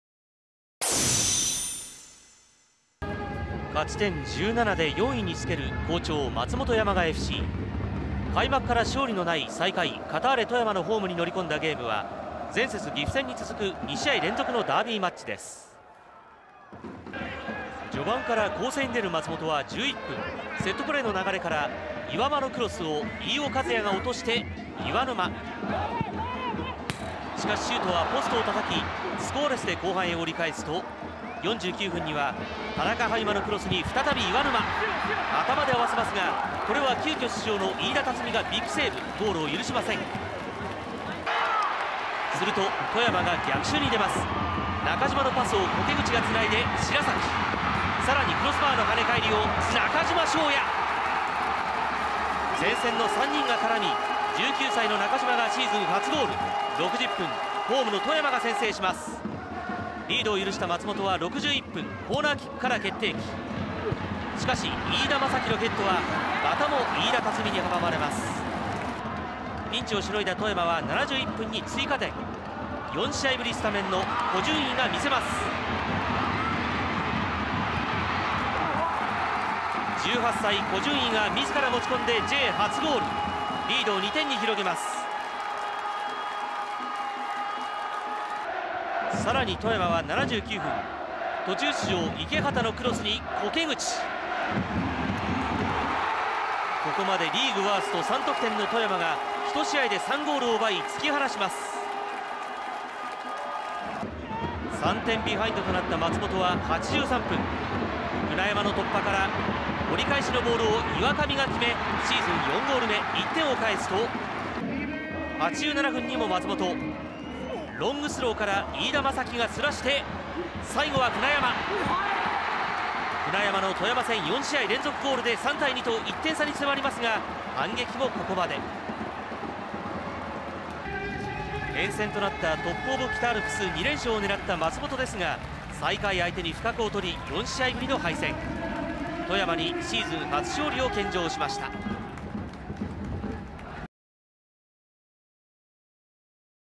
2014J2リーグ戦第10節「富山vs.松本」
2014J2リーグ戦第10節「カターレ富山vs.松本山雅FC」のスカパー！ハイライト動画です。
スカパー！はカターレ富山のリーグ戦を全試合生中継！！